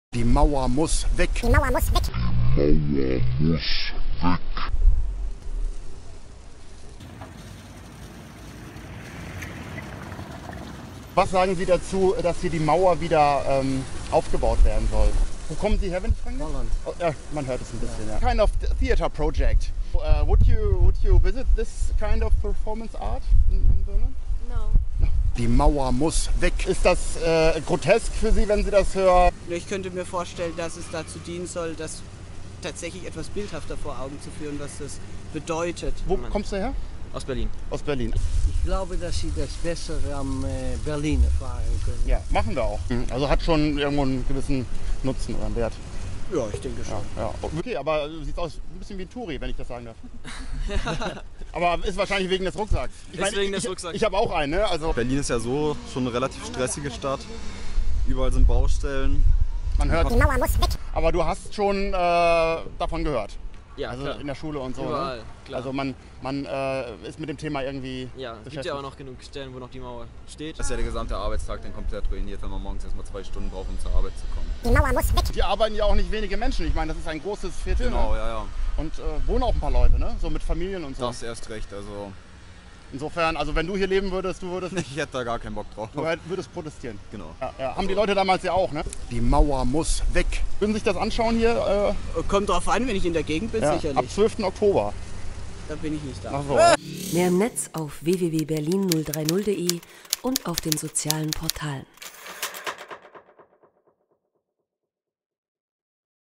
[030] vor Ort: Berliner Mauer wird wieder aufgebaut: DDR kommt zurück
Die Berliner Mauer wird wieder aufgebaut. Ein Theater-Regisseur will in Berlin-Mitte die DDR aufleben lassen. Ein totalitäres Regime mitten in Berlin? Ist das Kunst oder ist das Dreck? [030] war wieder vor Ort. Unser Kollege Felix Frerichs hat sich auf das Territorium der Diktatur begeben – und sich umgehört. Das sind die Reaktionen. Schaut es euch an.